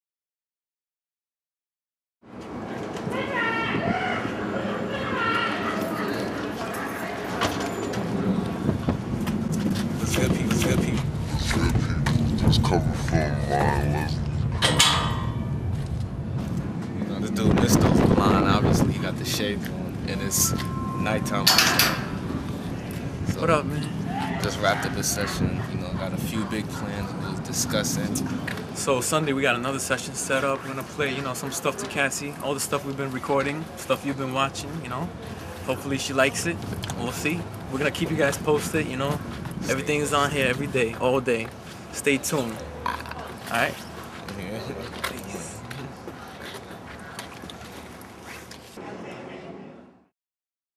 Mysto & Pizzi have a meeting with Ryan Leslie
Hey peoples, we spent half the day in traffic due to St. Patricks day traffic, it was terrible and we ended up being very late to our meeting with ryan, to our luck Ryan was running late as well due to traffic. We went over some plans and ideas for getting our music out there as well as set up the time for our session with Cassie on sunday night, make sure to stay tuned to our blog @ to see how the session went down and to see which tracks she picked.